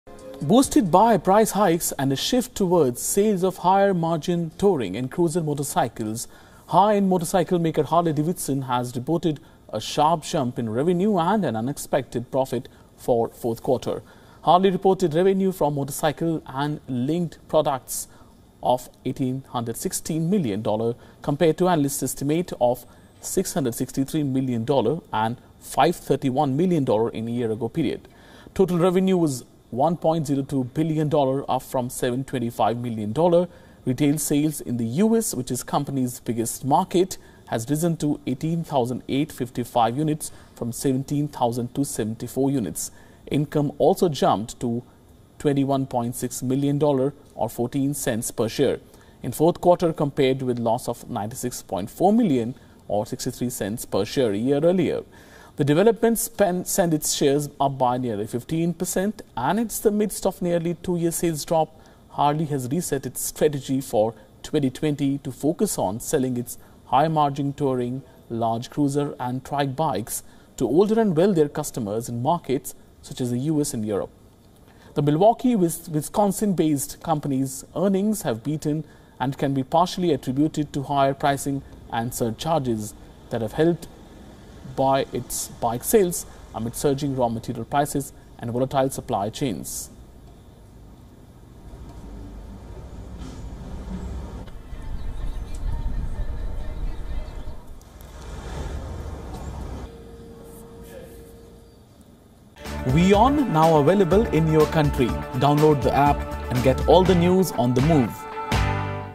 Harley-Davidson reports a sharp jump in revenue and unexpected profit for the fourth quarter | WION
Harley-Davidson reported a sharp jump in revenue and an unexpected profit for the fourth quarter, boosted by price hikes and a shift toward sales of higher-margin touring and cruiser motorcycles, sending its shares up nearly 15%.